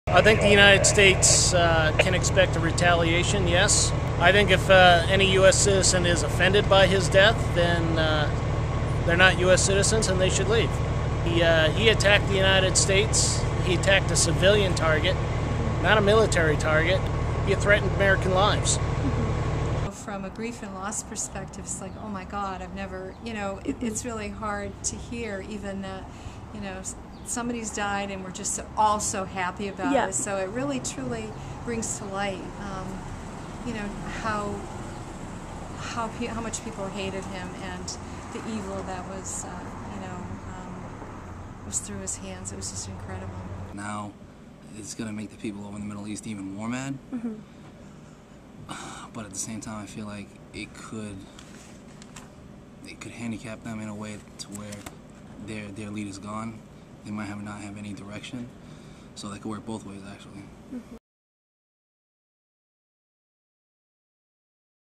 Local Reaction on the Death of bin Laden
Local reaction on the news of the death of Osama bin Laden.